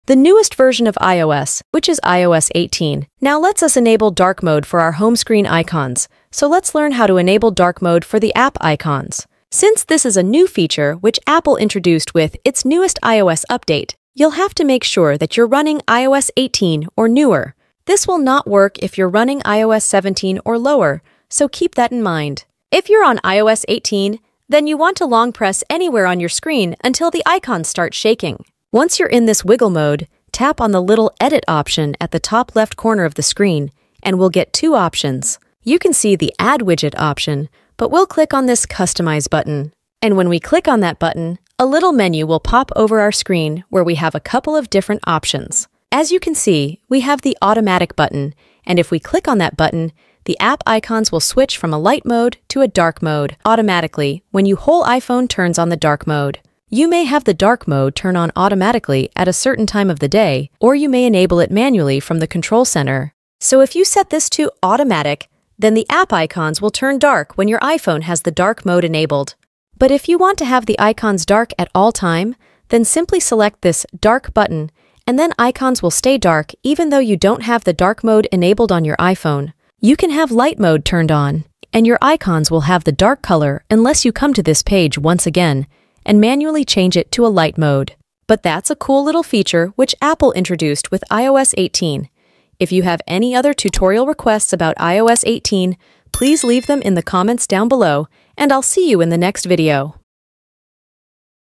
Enable DARK MODE For App Icons On iPhone iOS 18
In this video, we show you exactly how to enable dark mode for app icons on your iPhone running iOS 18. This is a new feature that will allow us to have dark mode for our app icons on our iPhone. If you're tired of the bright colored icons, then this video is for you!